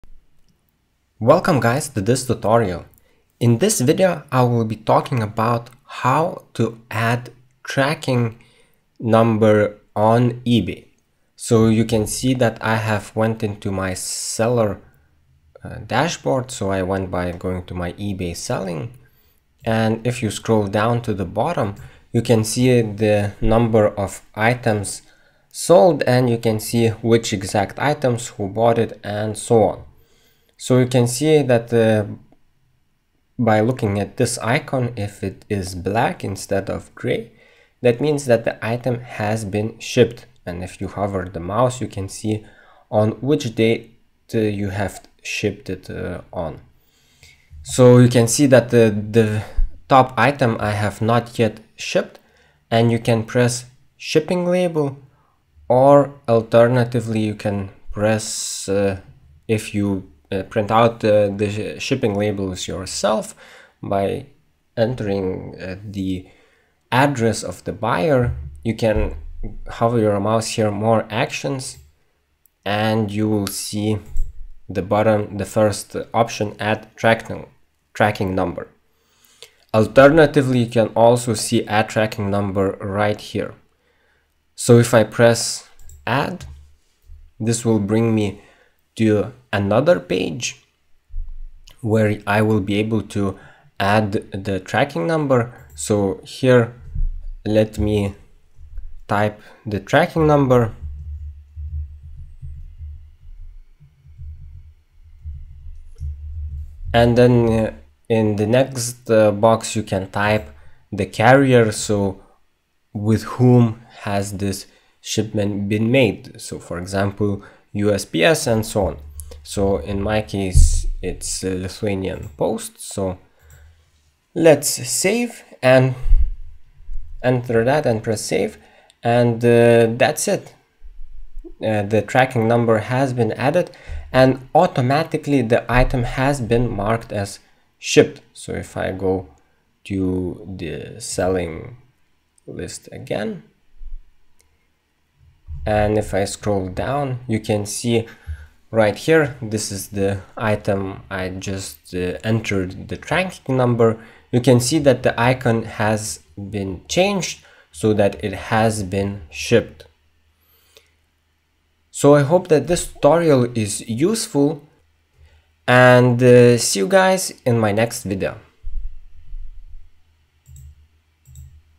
How to Add Tracking Number to eBay Shipped Item
In this video I show step by step how you can add a tracking ID number to the items that you have shipped out to customers on eBay.

⏱️TIMESTAMPS⏱️:
0:00 Introduction
0:11 Adding tracking number on eBay
2:40 Outro